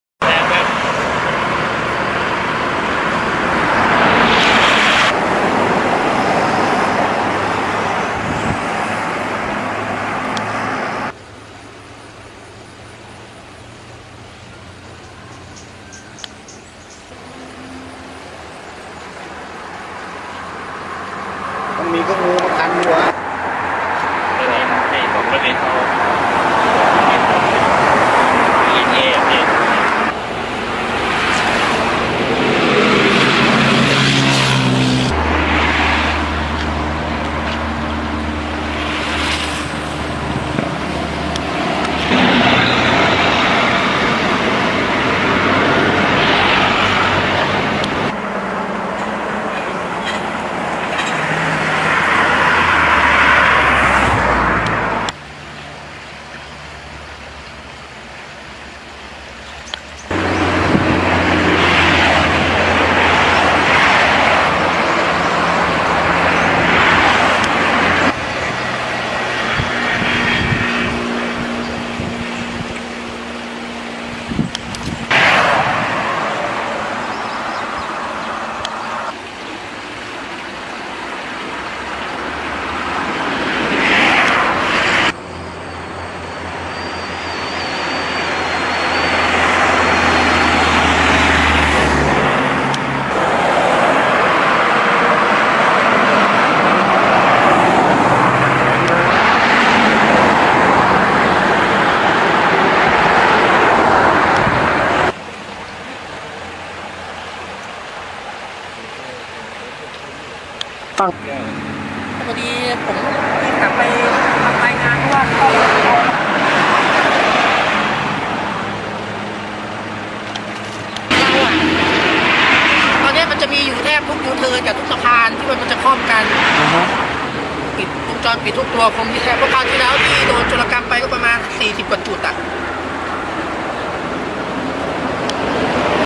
โจรแสบ!! ตัดสายไฟส่องสว่างใต้สะพานบ้านแก้ว ทำชาวบ้านจันทบุรีเดือดร้อน
096/10/2018 โจรแสบ!! ตัดสายไฟส่องสว่างใต้สะพานบ้านแก้ว ทำชาวบ้านจันทบุรีเดือดร้อน

จันทบุรี - โจรแสบ!! ลอบตัดสายไฟส่องสว่างที่ร้อยไว้ใต้สะพานบ้านแก้ว เขตเมืองจันทบุรี ทำชาวบ้านเดือดร้อนหนักเหตุไร้ไฟส่องสว่าง ขณะที่ หน.หมวดการทางหลวง เผยช่วง 2 เดือนในพื้นที่ 4 อำเภอ ถูกคนร้ายลอบตัดสายไฟเสียหายกว่า 5 แสนบาท